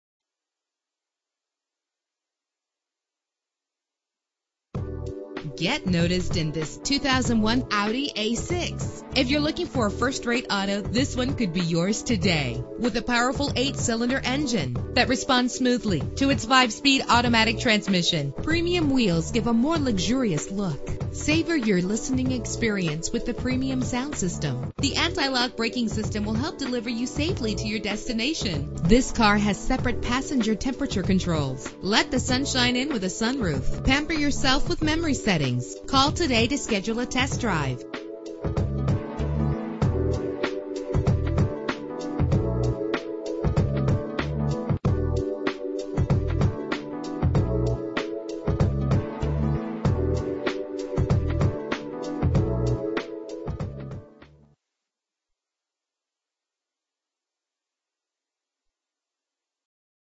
2001 Audi A6 - Atlanta GA
Year: 2001
Make: Audi
Model: A6
Trim: 4.2 quattro
Engine: 4.2 liter 8 cylinder 40 valve
Transmission: 5-Speed Automatic
Color: Blue Pearl
Mileage: 114530
Address:
5901 Peachtree Industrial Blvd